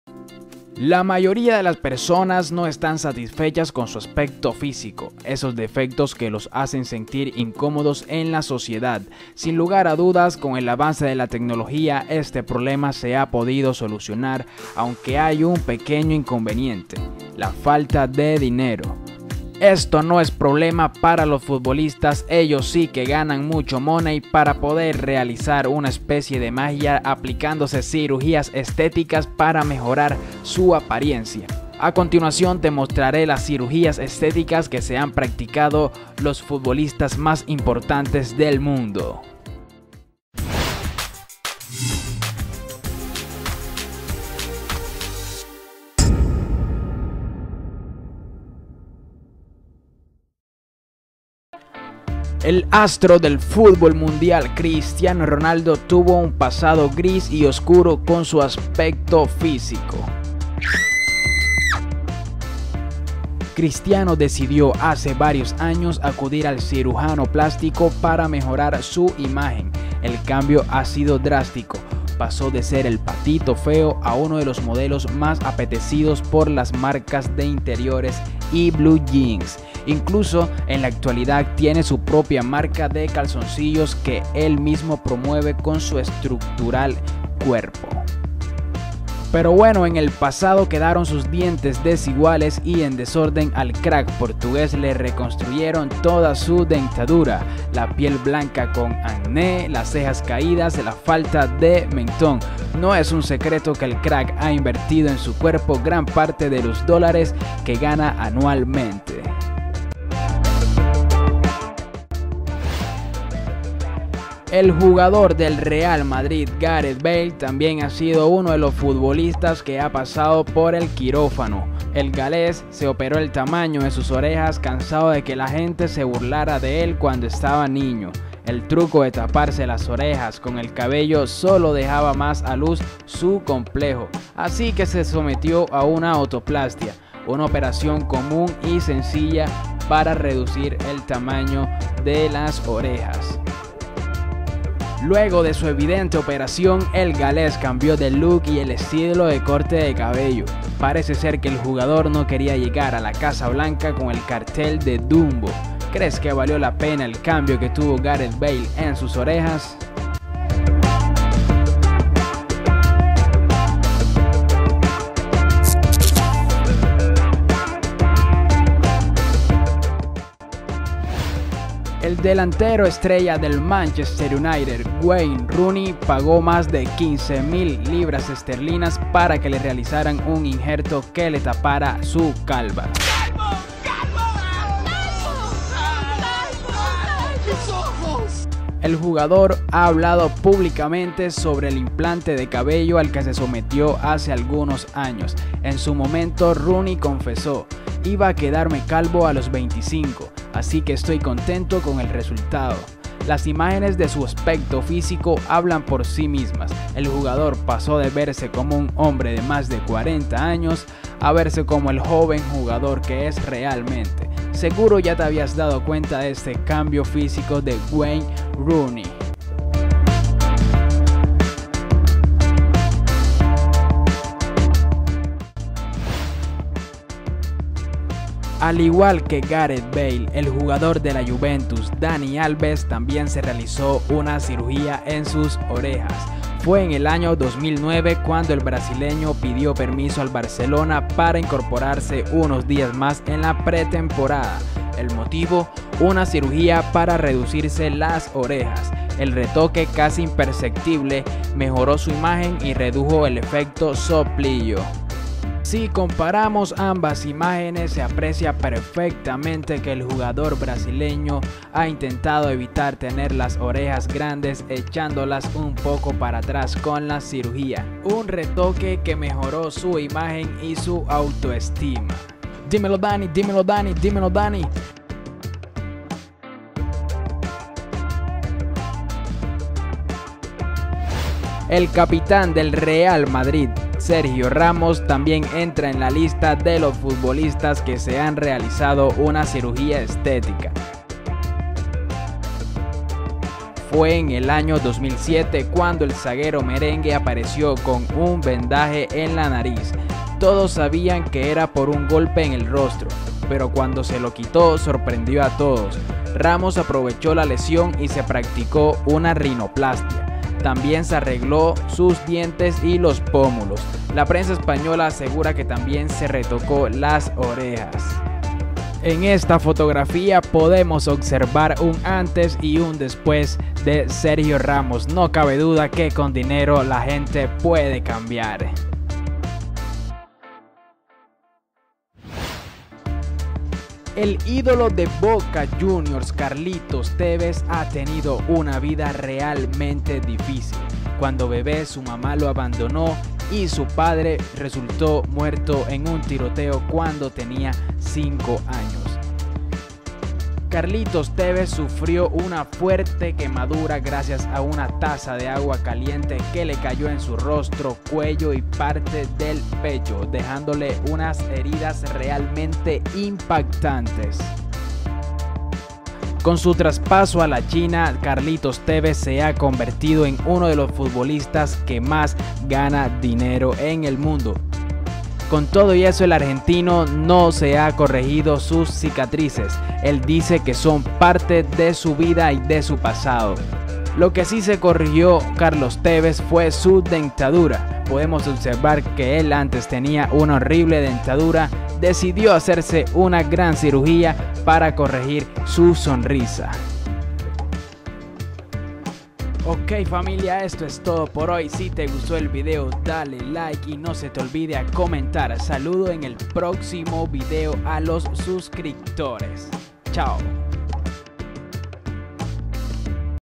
ASÍ FUERON LAS CIRUGÍAS ESTÉTICAS QUE SE HICIERON ESTOS FUTBOLISTAS. LOS CAMBIOS SON INCREÍBLES
Universal Camera presenta:
Cirugías estéticas que se hicieron estos futbolistas, los cambios son increíbles.

Cristiano ronaldo
Gareth bale
wayne rooney
dani alves
sergio ramos
carlos tevez

?? RECIBE NOTIFICACIONES CADA VEZ QUE PUBLICO UN NUEVO VÍDEO:
2. ? Dale Check a: "Recibir todas las notificaciones de este canal"
3. Dale a guardar